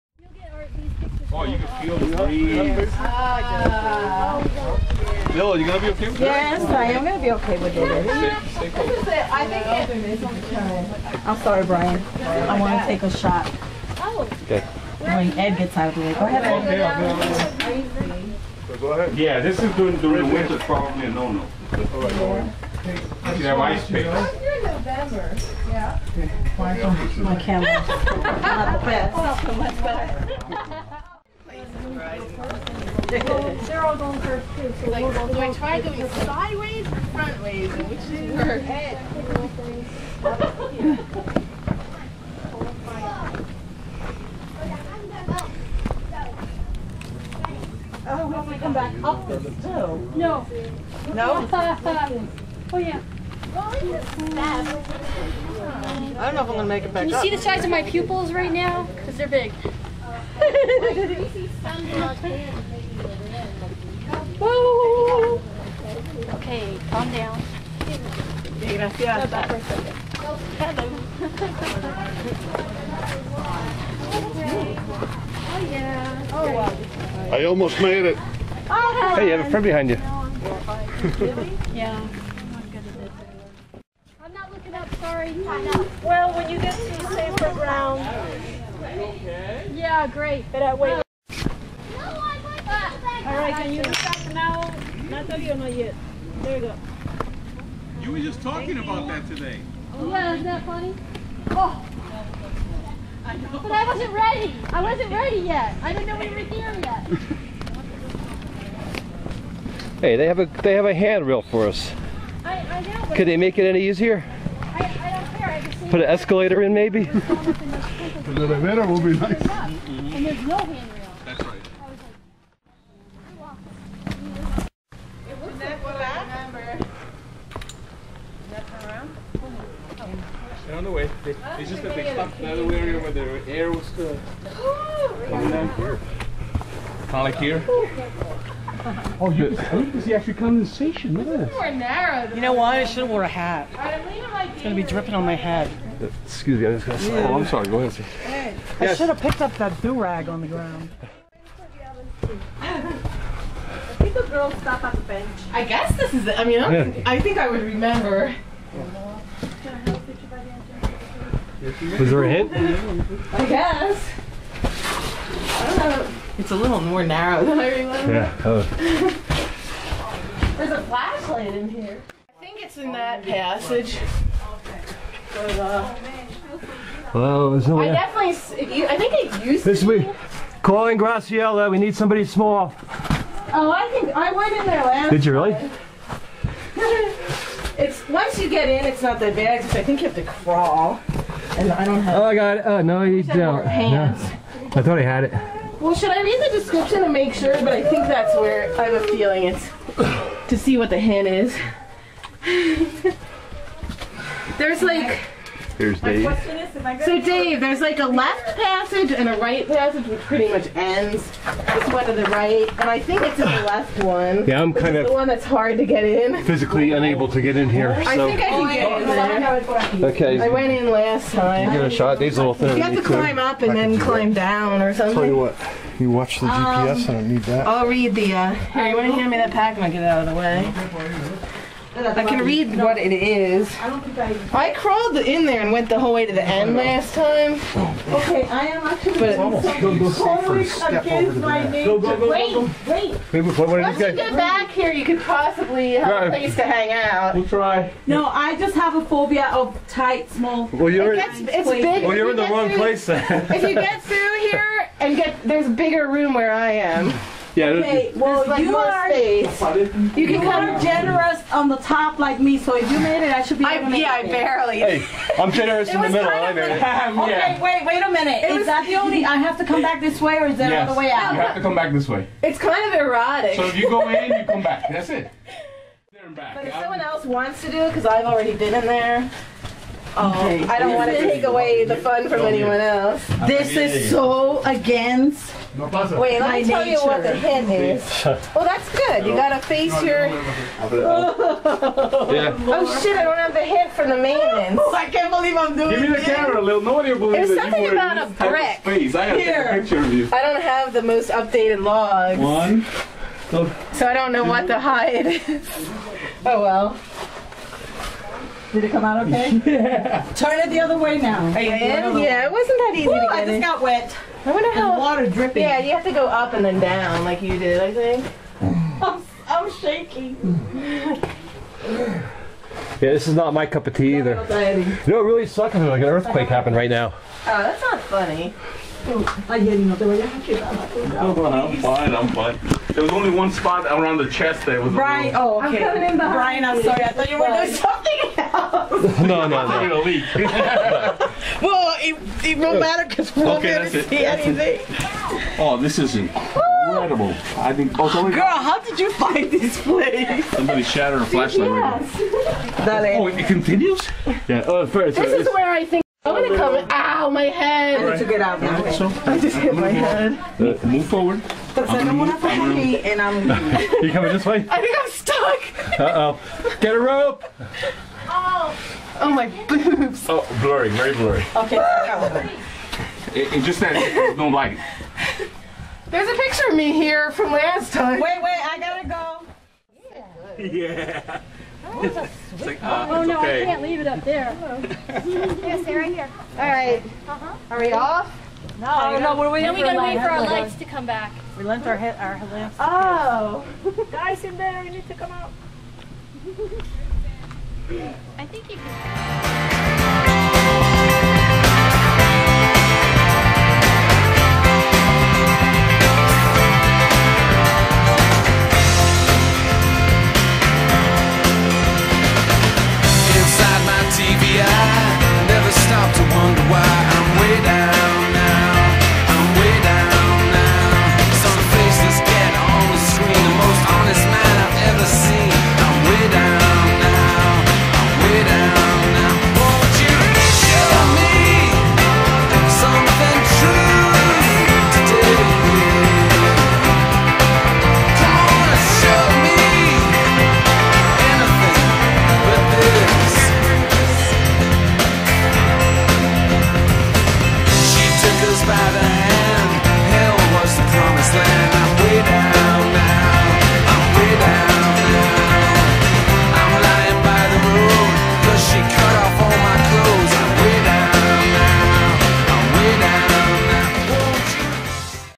Cornplanter's Cave Cache
We have some fun finding GCGY2V the day after Geowoodstock IX.